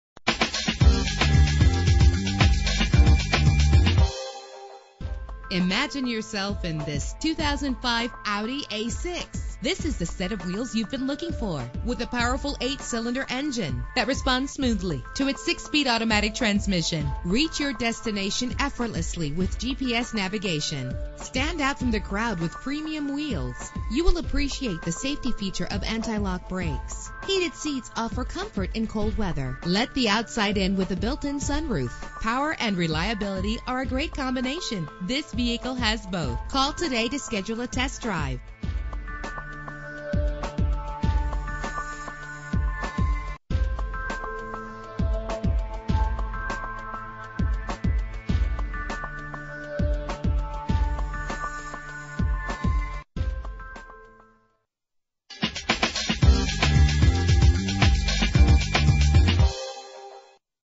2005 Audi A6 - Richardson TX
Year: 2005
Make: Audi
Model: A6
Trim: 4.2 quattro
Engine: 4.2 liter 8 cylinder 40 valve
Transmission: 6-Speed Automatic
Color: Blue
Mileage: 56682
Address:
300 N. Central Expressway